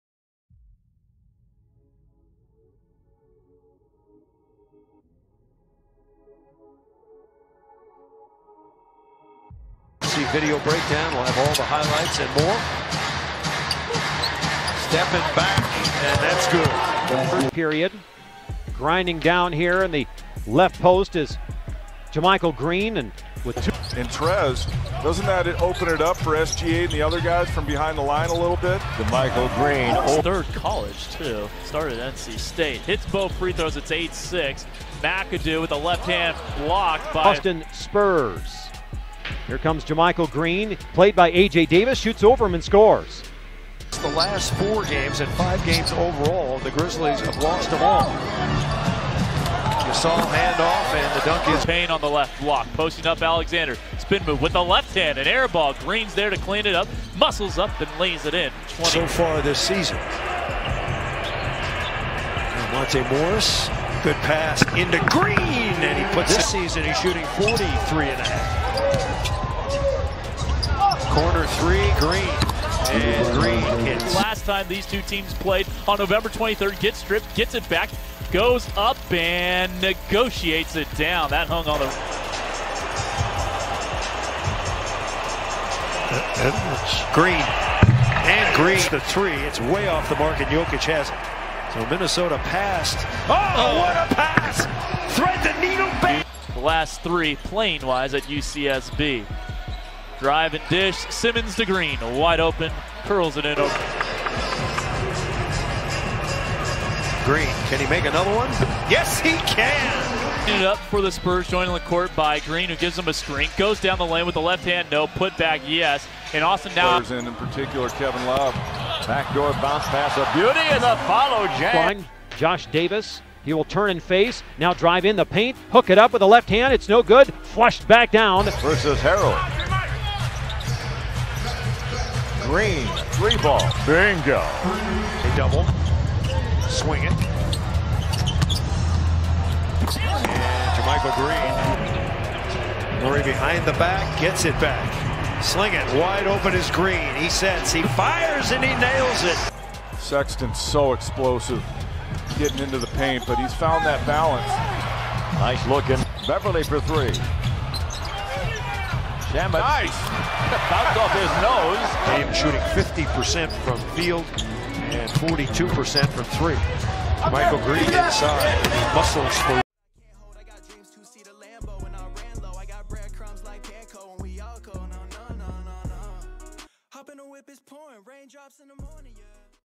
OKLAHOMA CITY THUNDER: JaMychal Green ᴴᴰ
BREAKING NEWS || OKLAHOMA CITY THUNDER || JAMYCHAL GREEN HIGHLIGHTS
487 CAREER GAMES
7.9 POINTS PER GAME
5.7 REBOUNDS PER GAME
0.9 ASSISTS PER GAME